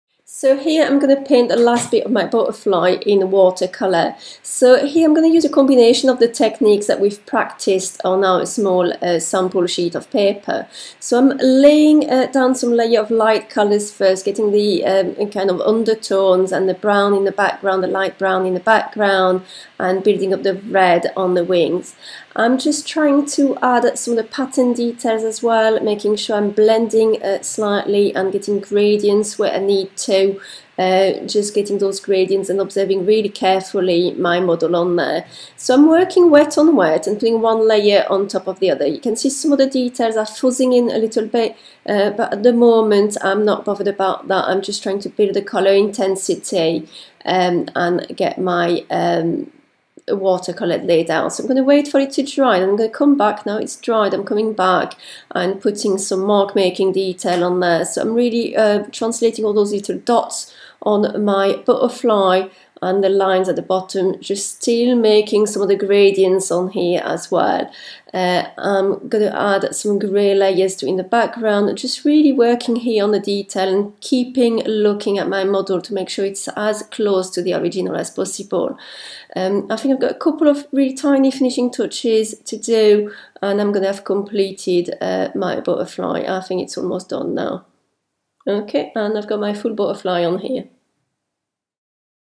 Painting a Butterfly in Watercolour
Learn to paint a butterfly in watercolour using a range of techniques to build colour intensity.